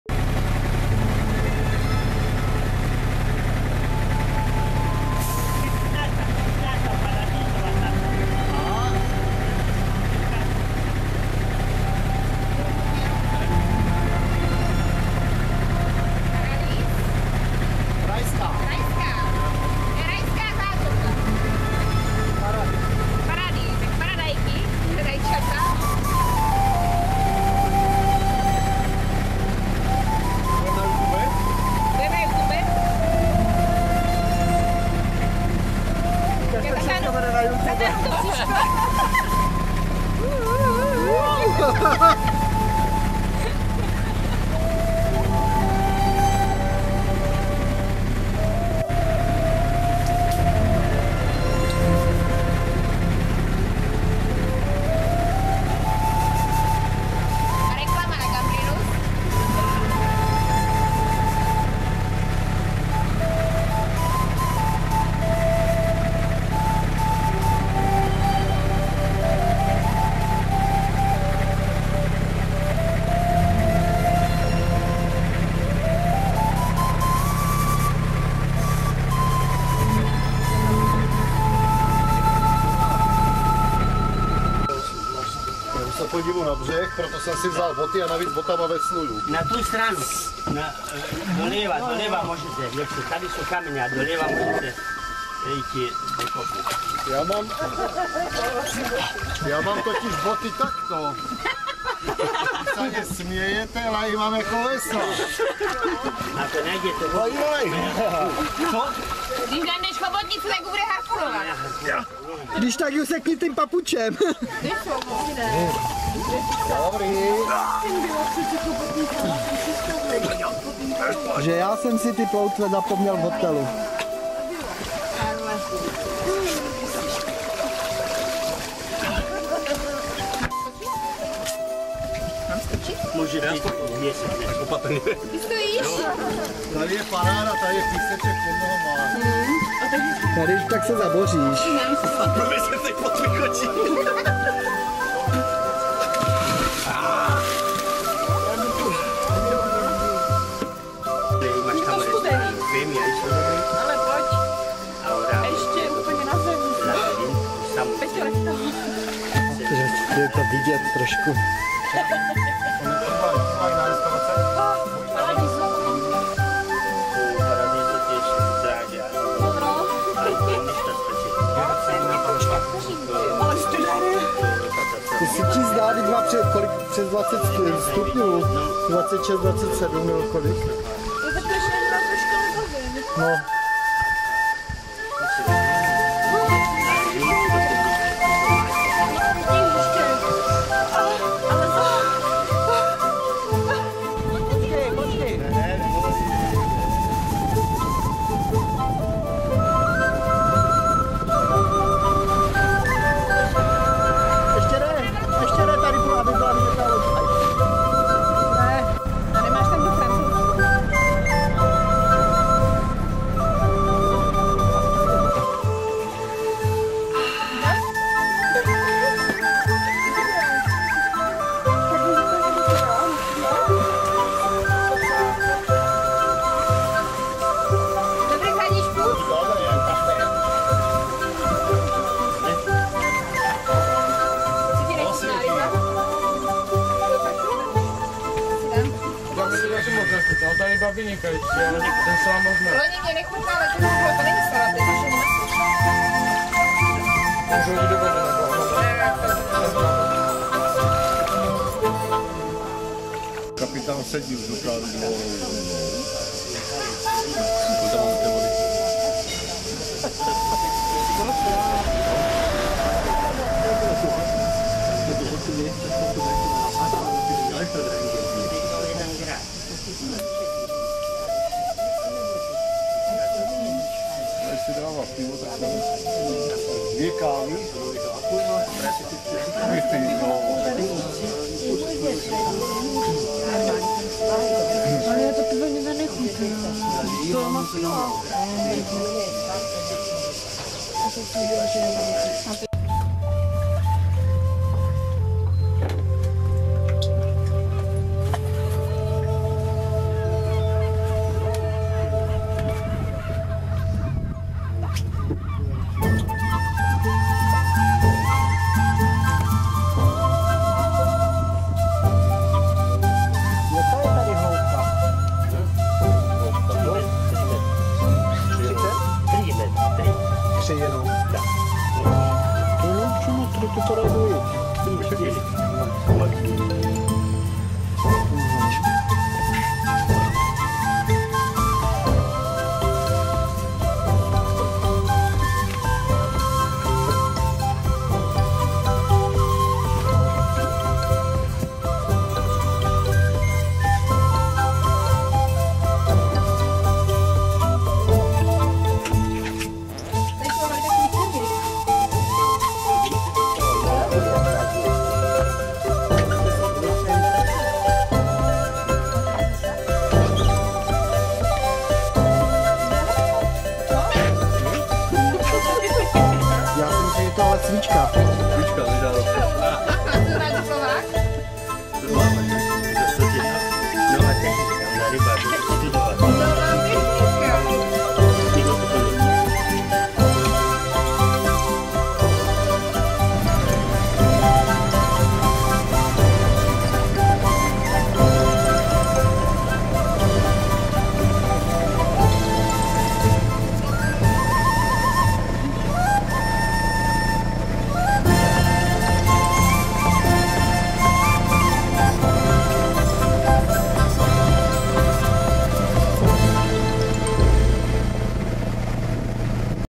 okolo otoka Rab - Pag
Plavba okolo ostrova Rab s koupáním a obědem na ostrově Pag